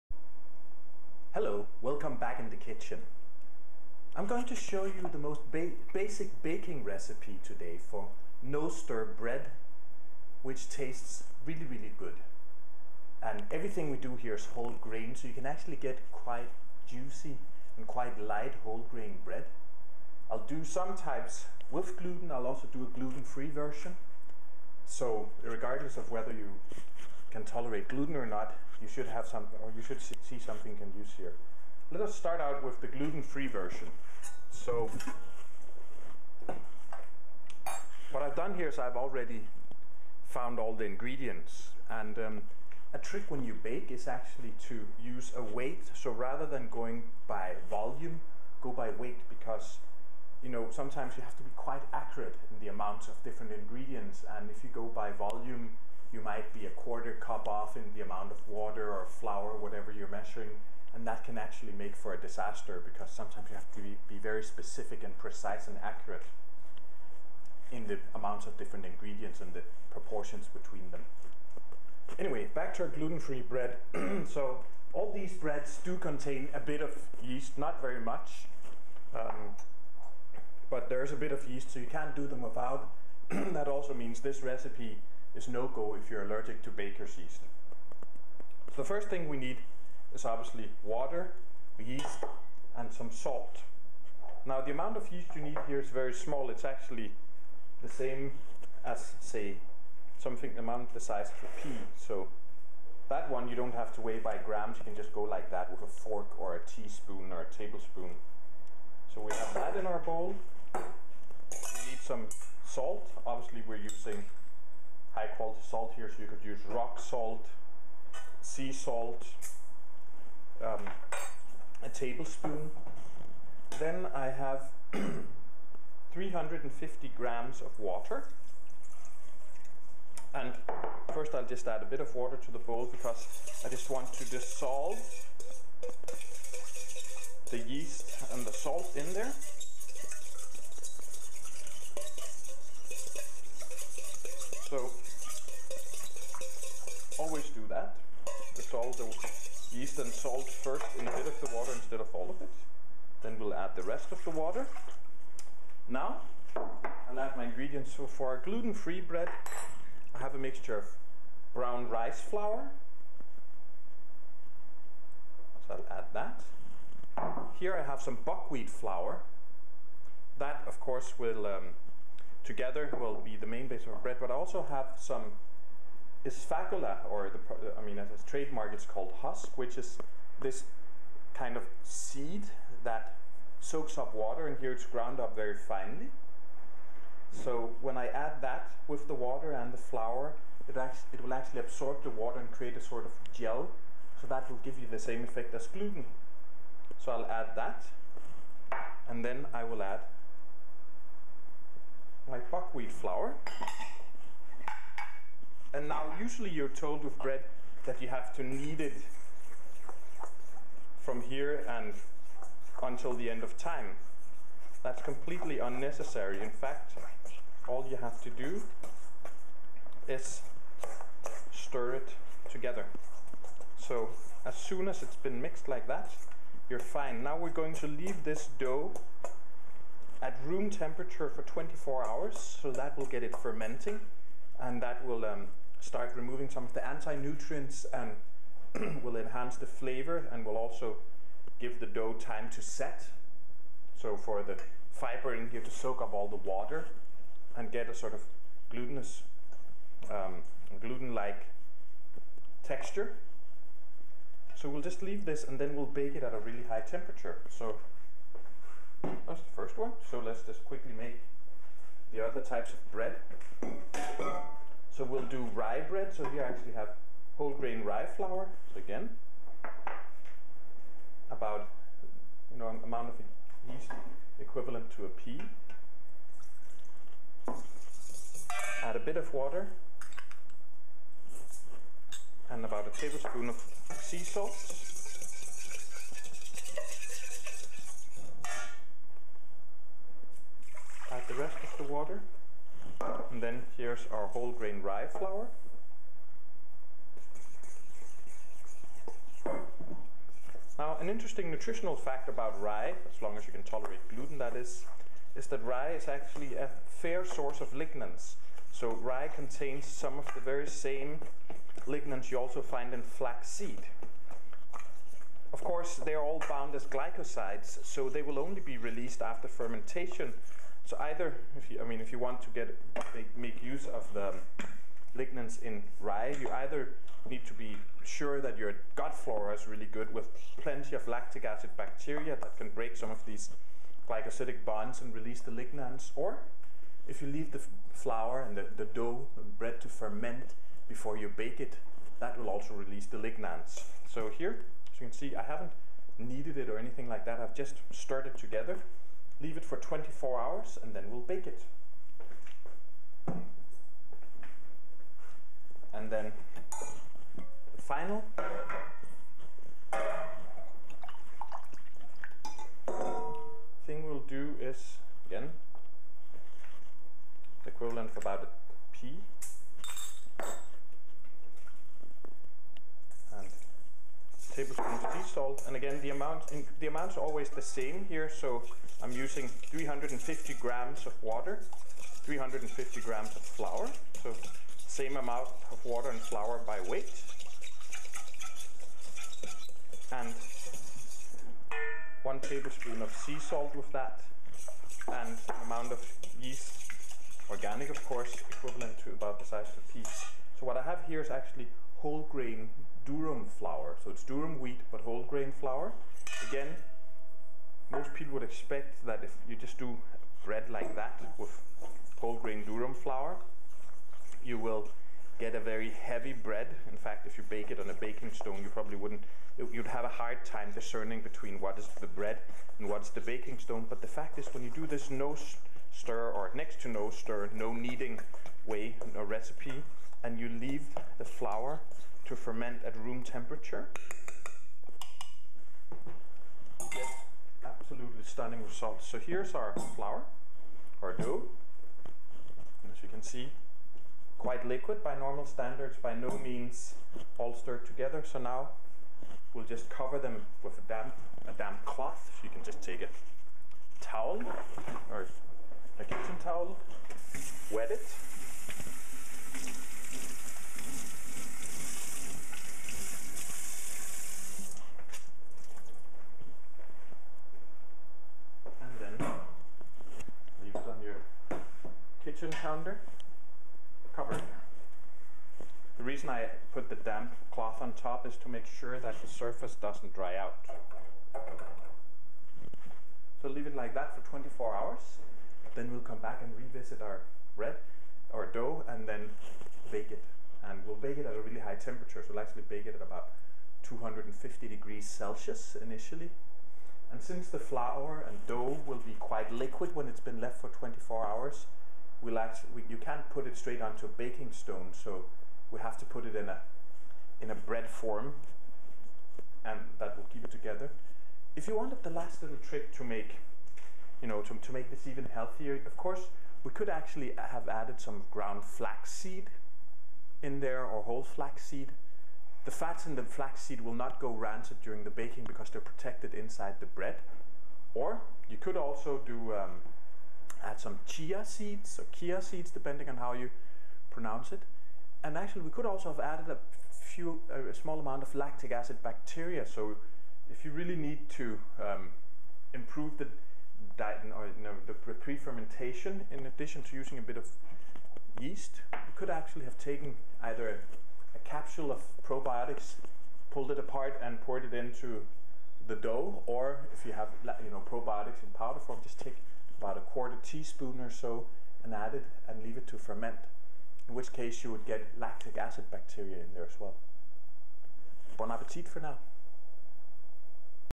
No-Knead Bread With and Without Gluten
How to make and bake bread that you do not knead. With and without gluten. For the "Supplementation and Whole Food Nutrition" course in The Master of Science In Nutrition and Functional Medicine at UWS.